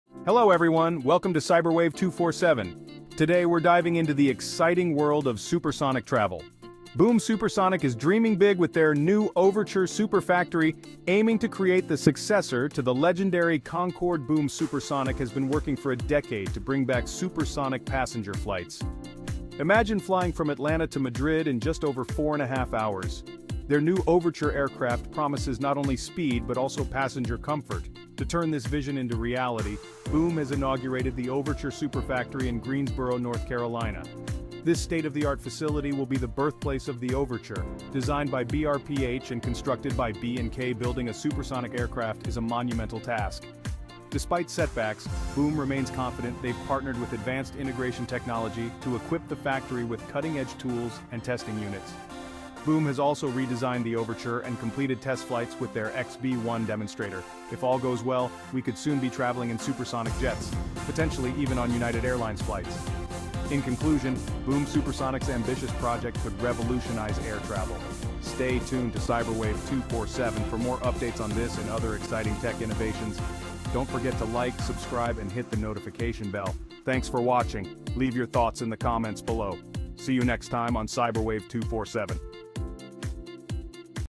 🚀 Boom Supersonic: The Future of Air Travel ✈️
Welcome to CyberWave247! 🌐 Dive into the exciting world of supersonic travel as Boom Supersonic aims to revive the glory of the Concorde with their new Overture Superfactory. Learn about their ambitious plans, the state-of-the-art facility in Greensboro, North Carolina, and the challenges they face in bringing supersonic passenger flights back. Will we soon be flying from Atlanta to Madrid in just over four hours? Find out in this video!